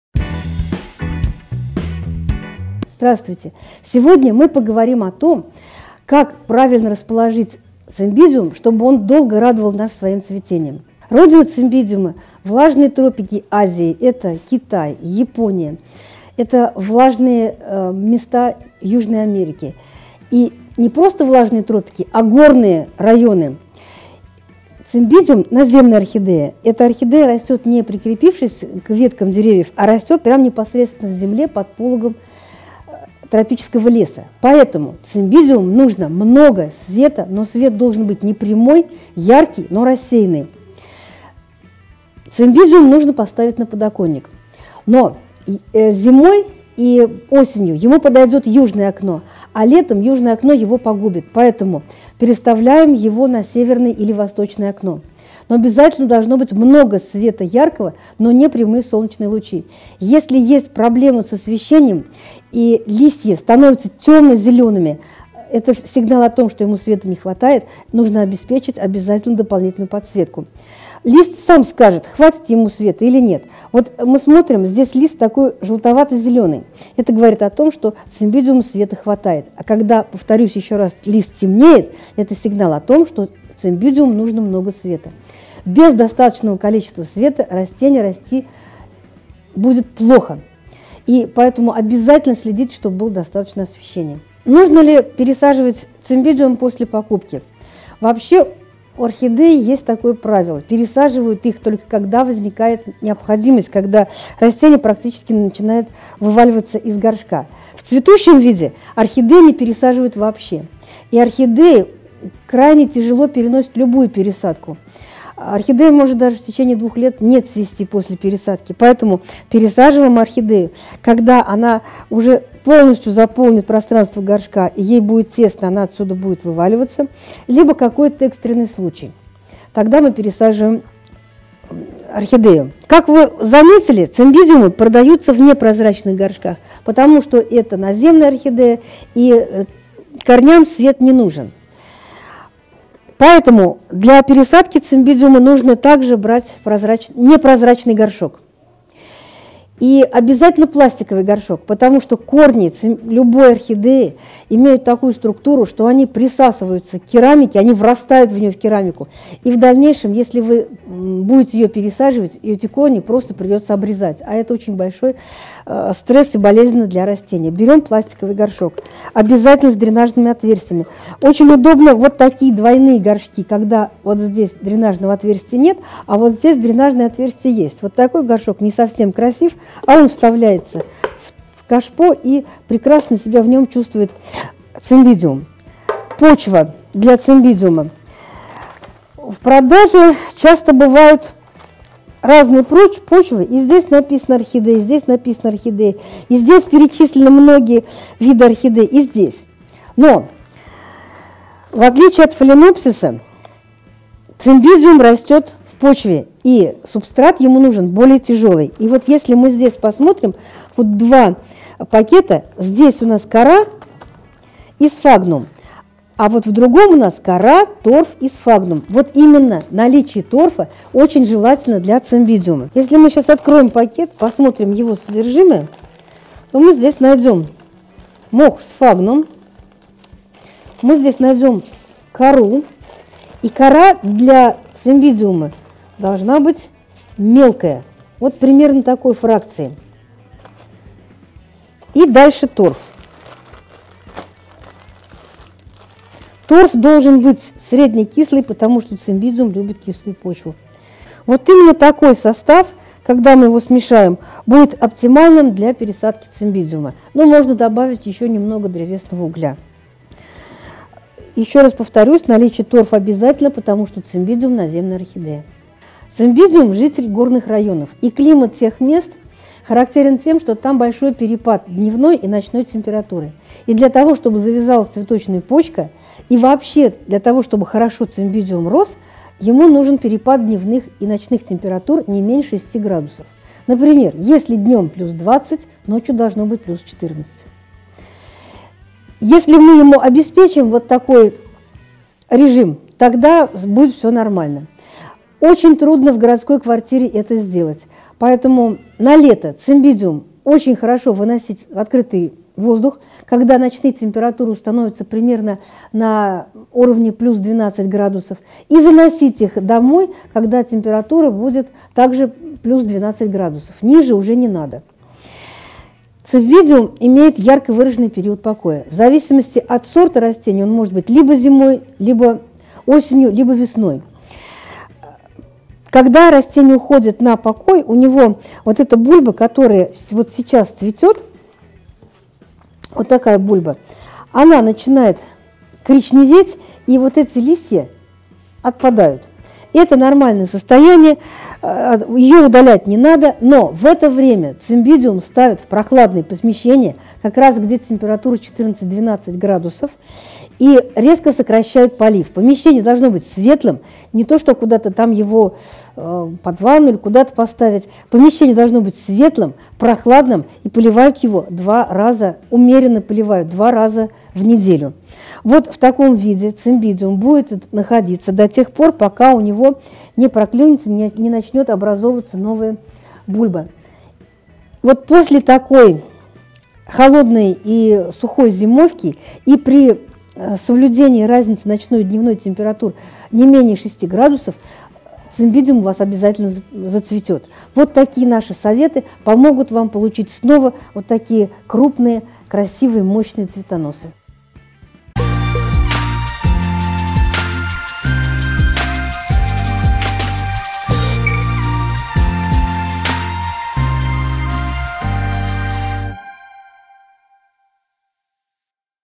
Орхидея Цимбидиум 🌼 Как правильно ухаживать за цветком ✅ Часть 2
Расскажем как ухаживать за Орхидеей Цимбидиум. Как правильно поливать, пересаживать. Чего ни в коем случае нельзя делать при выращивании орхидеи.